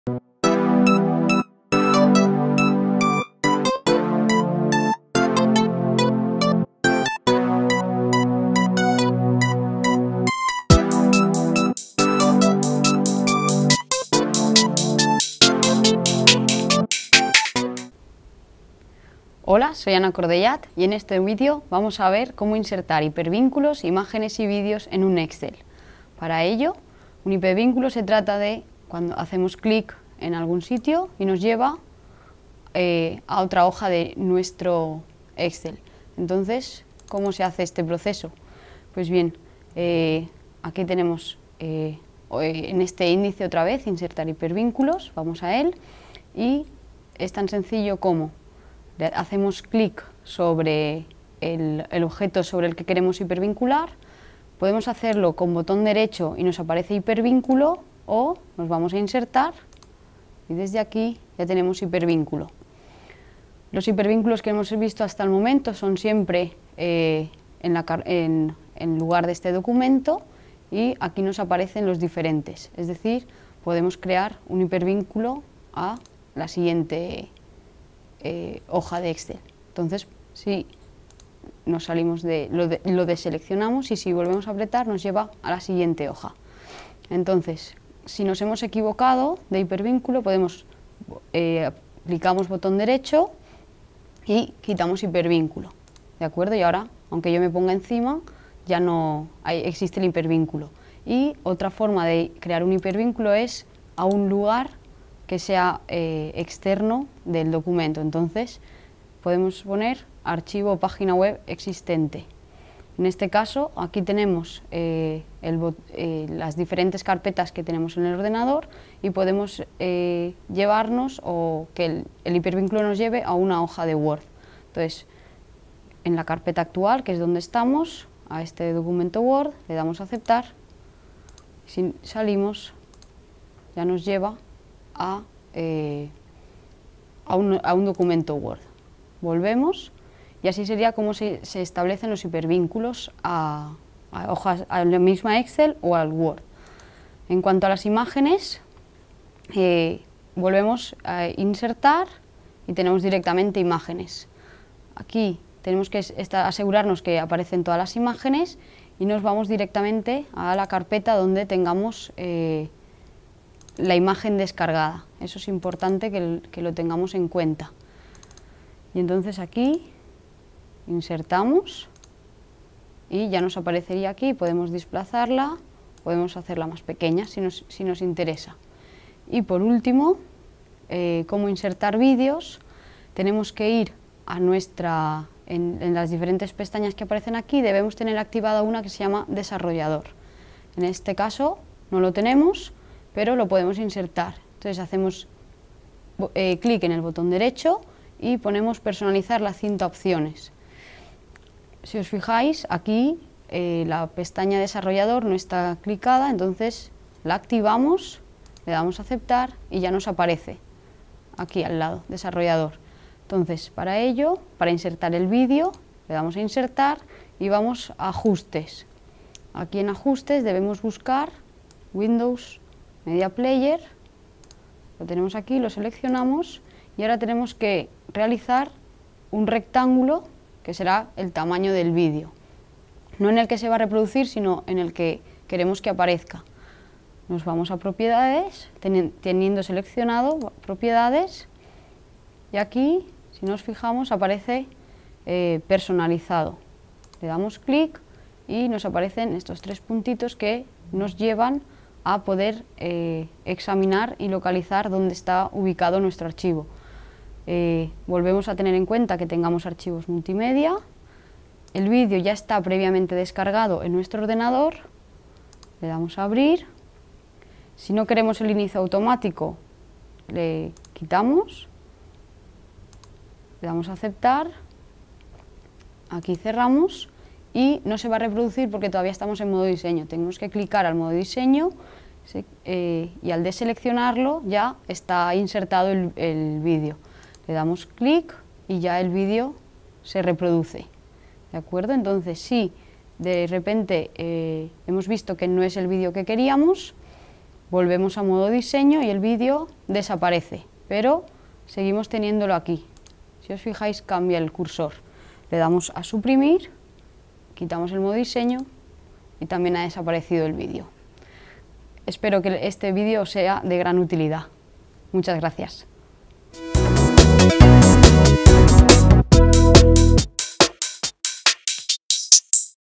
Insertar hipervínculos, imágenes y vídeos en Excel
"Autor: Cordellat Marzal, Ana
Serie: PÍLDORAS DE CONOCIMIENTO EN LA CREACIÓN DE TRABAJOS ACADÉMICOS
Año: 2019
Resumen: Poder insertar información desde otros lugares puede ser interesante para aumentar la calidad de los trabajos. Es por ello, que aquí vamos a ver cómo podemos insertar hipervínculos, imágenes y vídeos.